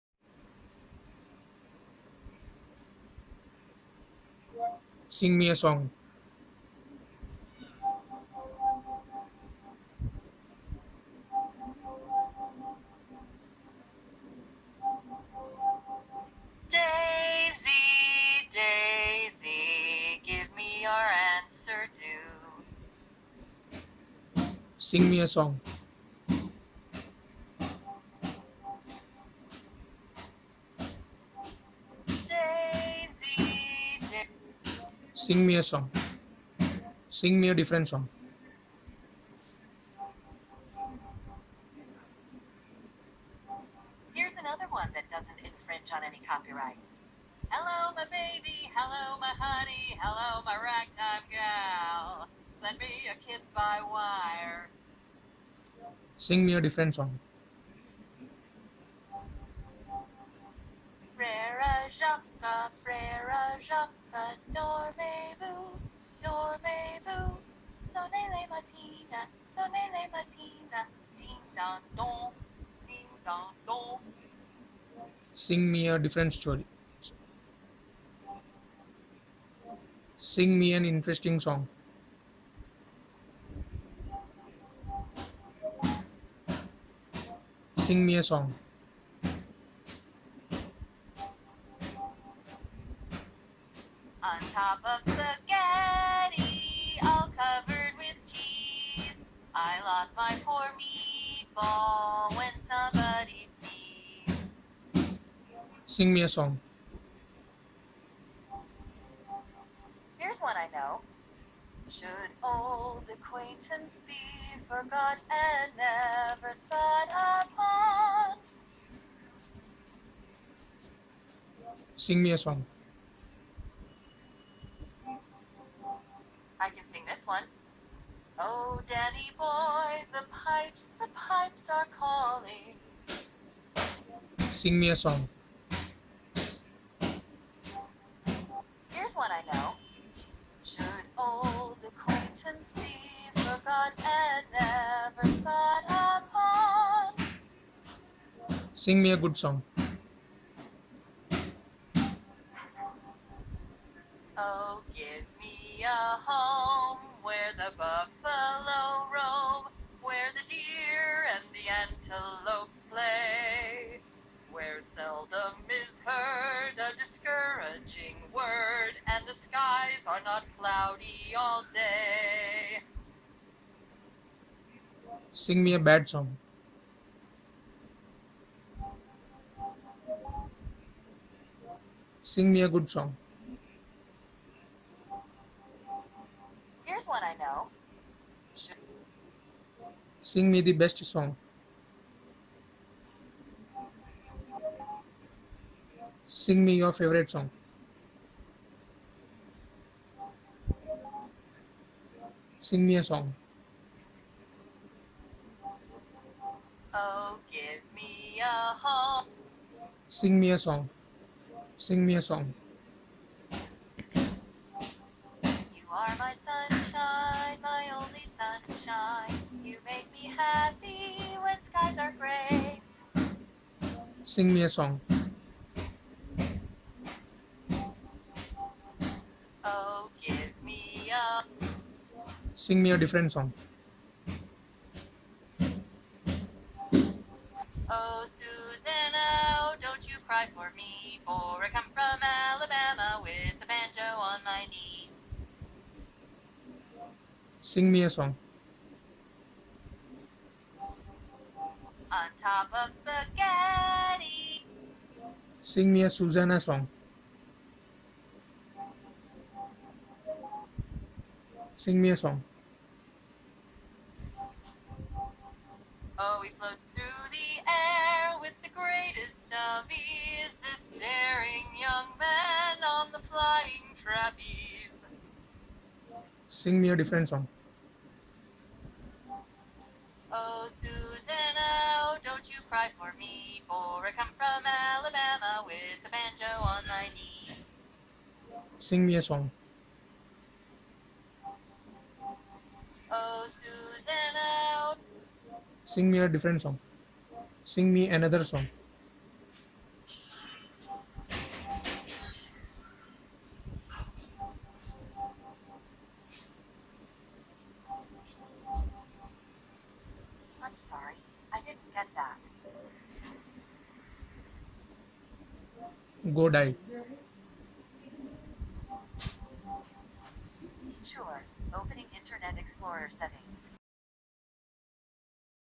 170) Cortana songs compilation on lumia 640
here is a compilation of songs that cortana sings when asked to.this video was shot on my nokia lumia 520.